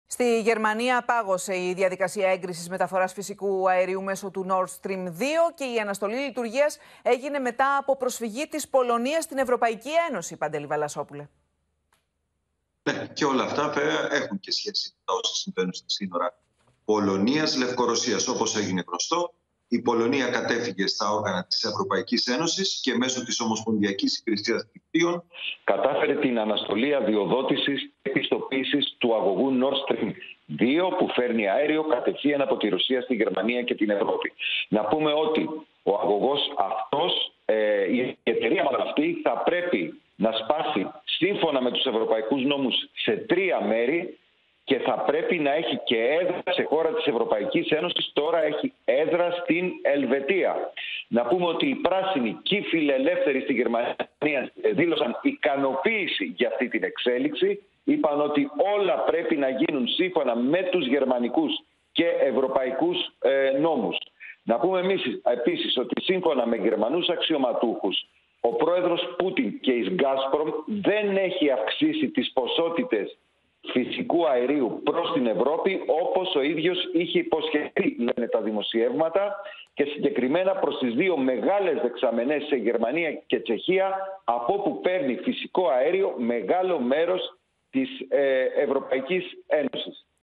Με παρέμβαση της Πολωνίας το γερμανικό «όχι» για τον Nord Stream 2 | Μεσημεριανό Δελτίο Ειδήσεων
Στη Γερμανία πάγωσε η διαδικασία έγκρισης μεταφοράς φυσικού αερίου μέσω του Nord Stream 2 και η αναστολή λειτουργίας έγινε μετά από προσφυγή της Πολωνίας στην Ε.Ε.